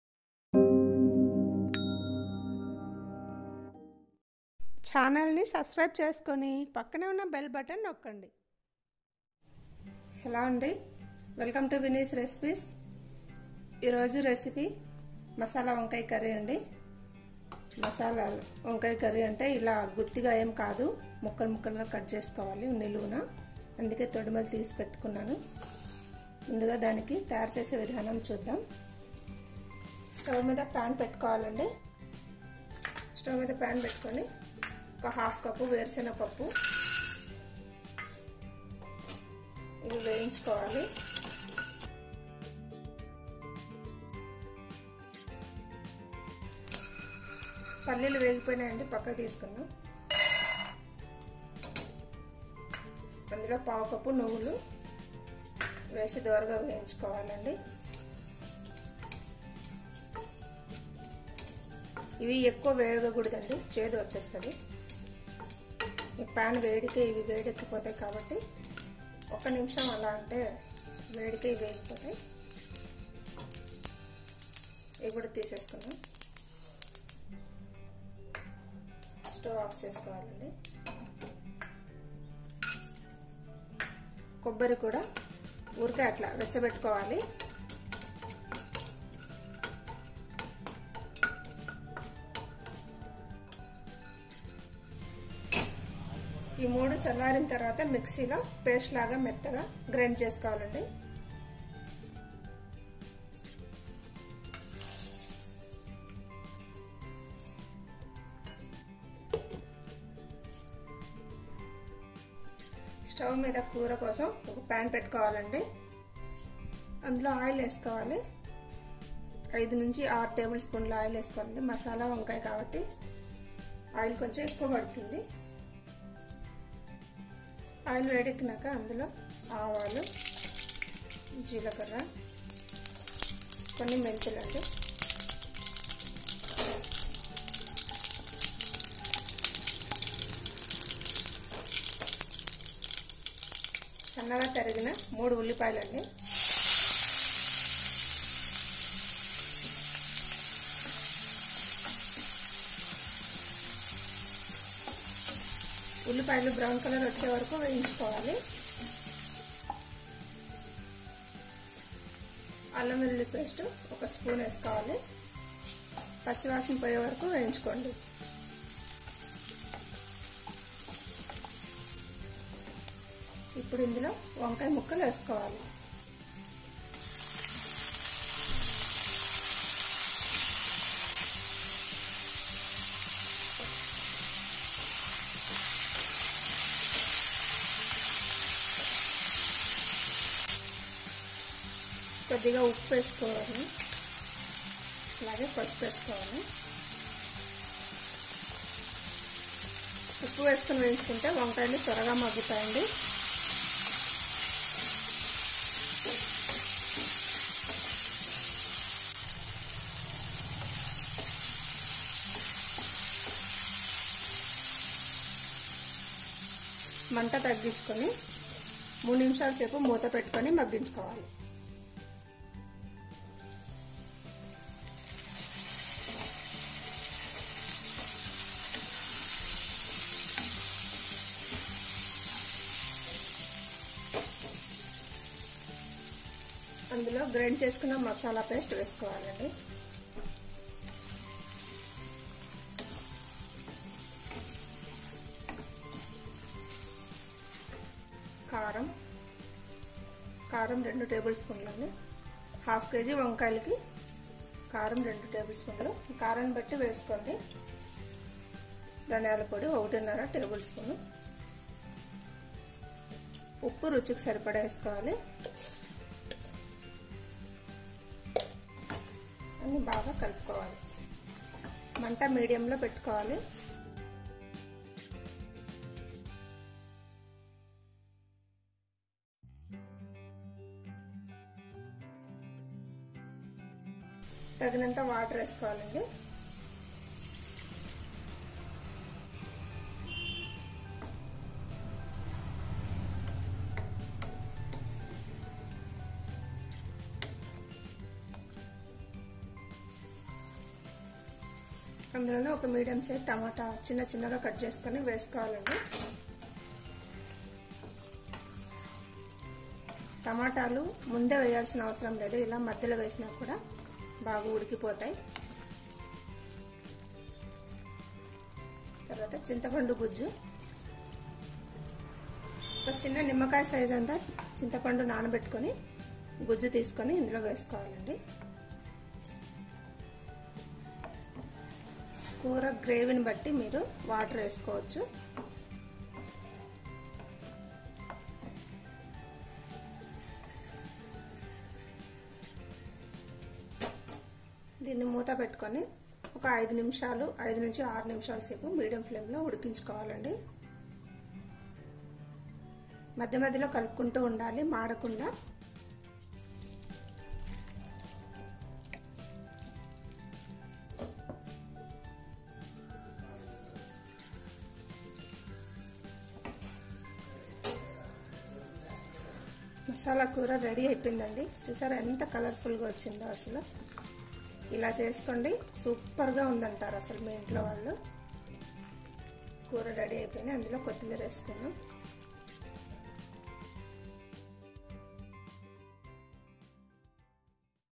మాసాల వంకాయ | Brinjal masala curry | ఆంధ్ర style Gutti vankaya
How to make andhra style వంకాయ మాసాల curry